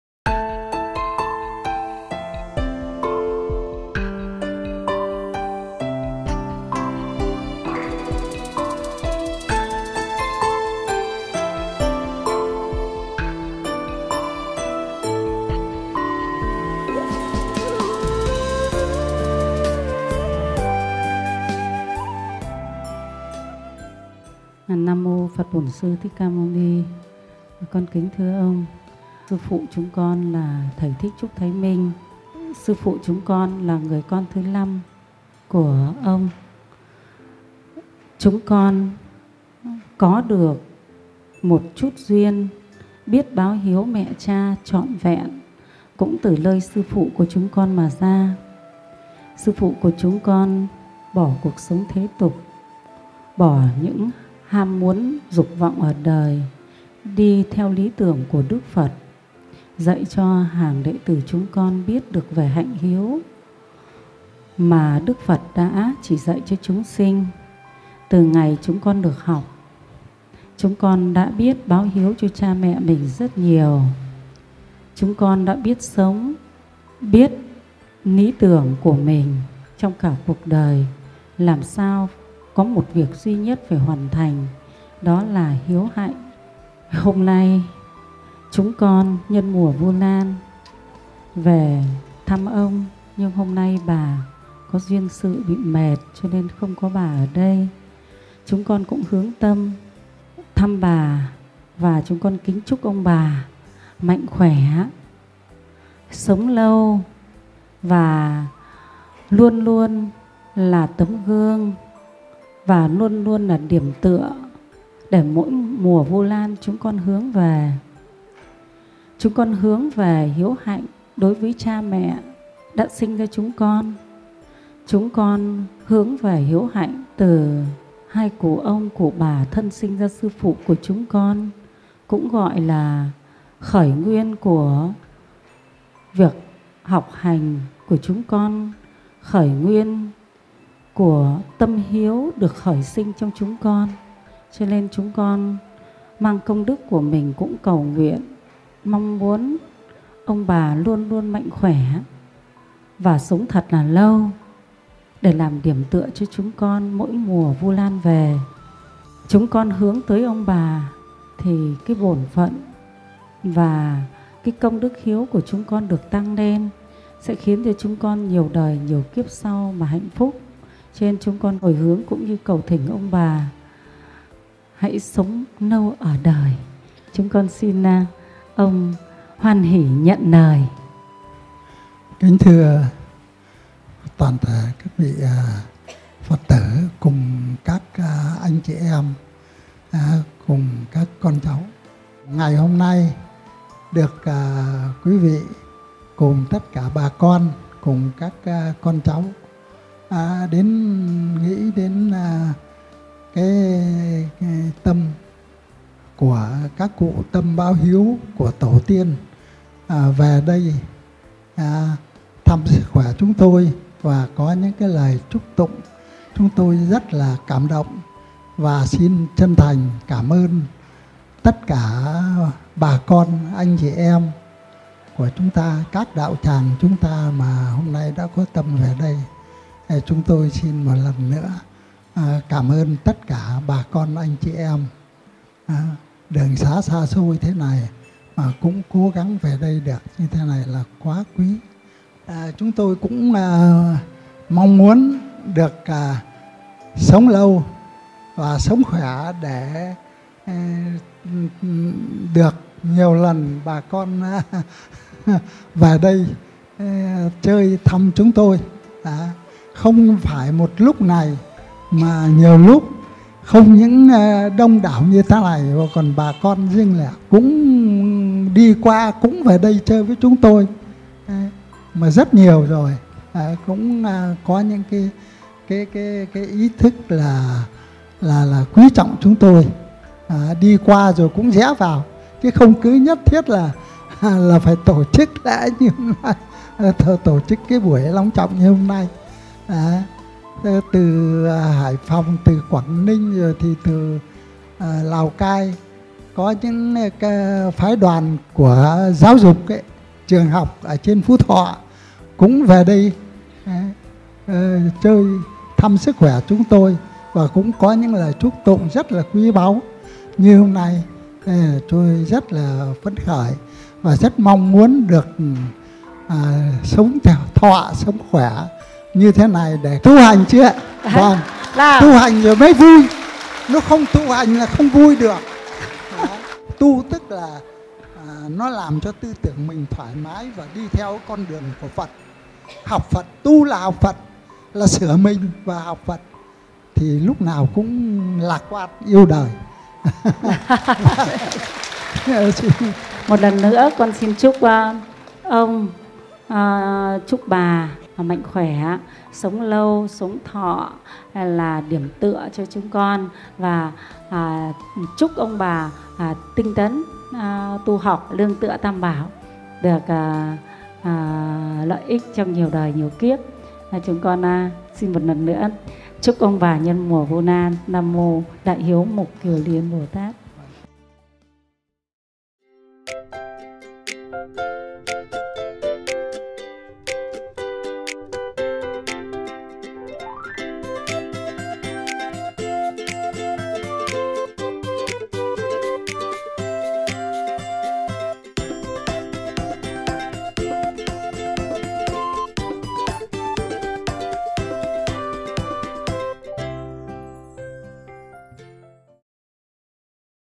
Tri ân hai cụ thân sinh Sư Phụ Thích Trúc Thái Minh - Nguồn khởi sinh tâm hiếu
Phật tử Phạm Thị Yến - Chủ nhiệm CLB Cúc Vàng - Tập Tu Lục Hoà chia sẻ với các Phật tử trong CLB.
"Nhân mùa Vu lan, chúng con kính chúc ông bà sống lâu và luôn là tấm gương và điểm tựa để mỗi mùa Vu lan chúng con hướng về. Chúng con hướng về hiếu hạnh đối với cha mẹ đã sinh ra mình và hướng về hiếu hạnh từ nơi ông bà thân sinh ra Sư Phụ của chúng con, là khởi nguyên của việc học Pháp và tâm hiếu trong mỗi chúng con. Chúng con nguyện mang công đức của mình mong nguyện ông bà luôn luôn mạnh khỏe và sống lâu để làm điểm tựa cho chúng con mỗi mùa Vu lan về."
_Phạm Thị Yến (Tâm Chiếu Hoàn Quán)_
Kính mời Quý đạo hữu xem thêm các bài theo các chủ đề khác:
*** Hướng dẫn tu tập cầu siêu hóa giải oán kết (bài 8)
Kính mời Quý đạo hữu tham gia các nhóm và các trang để được giải đáp thắc mắc: